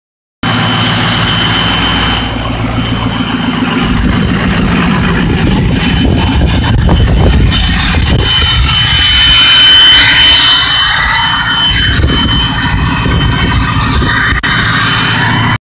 Powerstroke turbo whistle
My powerstroke with Garrett stage one during high idle